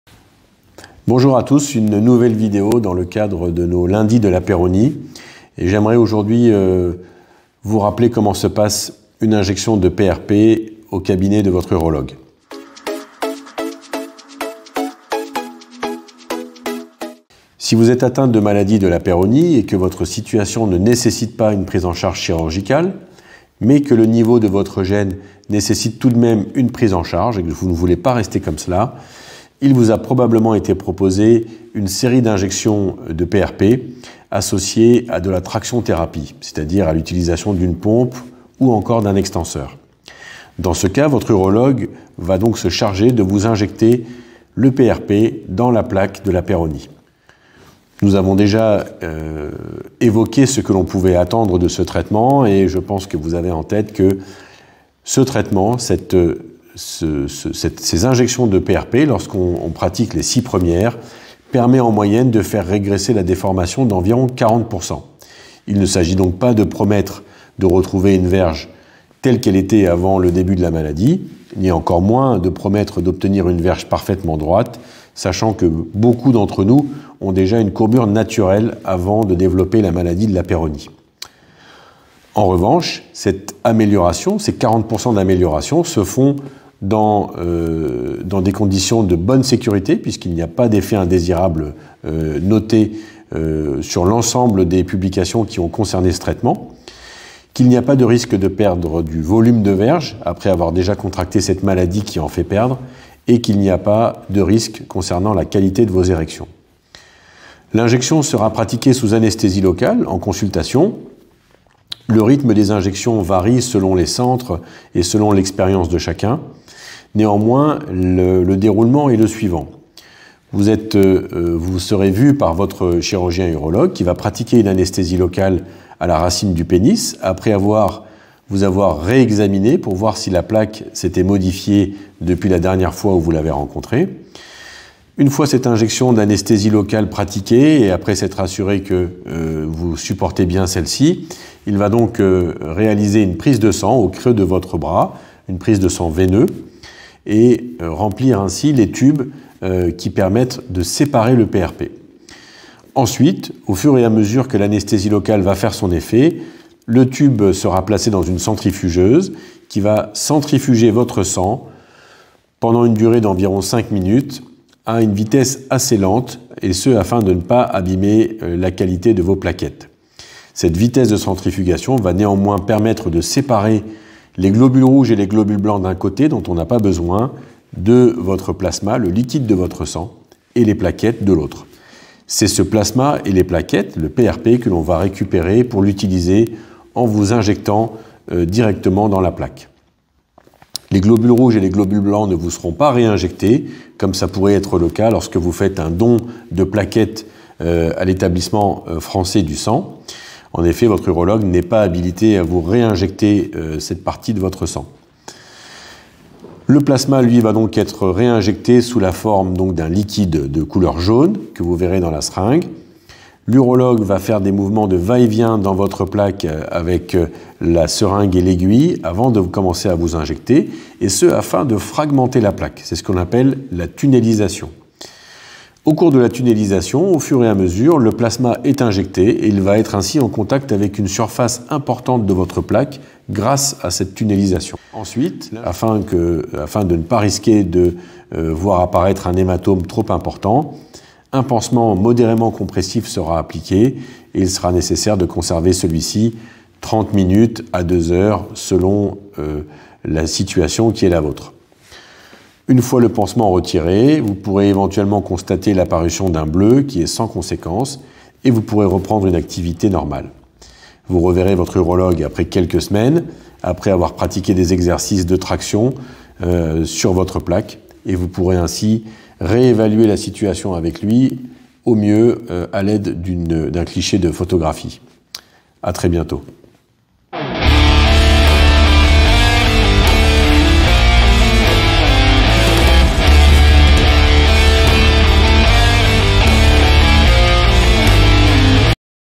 Injection de PRP
Si vous êtes atteints de maladie de Lapeyronie et que votre situation ne nécessite pas une prise en charge chirurgicale, on vous a peut-être proposé une série d'injections de PRP associée à de la traction thérapie. Dans cette vidéo de la série des Lundis de Lapeyronie, le Dr. Beley vous explique comment se déroule une injection de PRP (Plasma riche en plaquette) à partir d'une simple prise de sang de 10ml réalisée en cabinet.